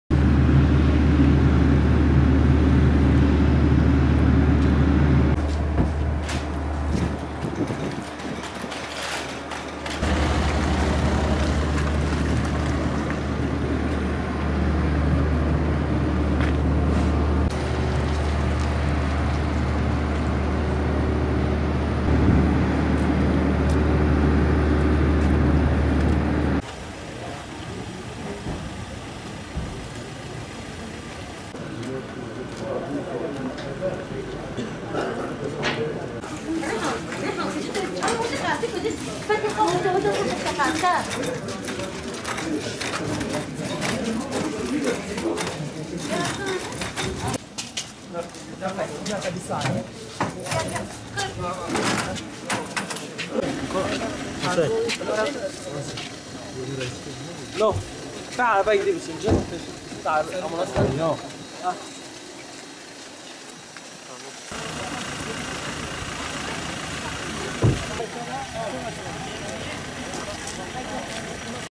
Silopi’de termik santralde patlama: 3 yaralı
Şırnak’ın Silopi ilçesinde özel bir şirkete ait termik santralde meydana gelen patlamada 3 kişi yaralandı.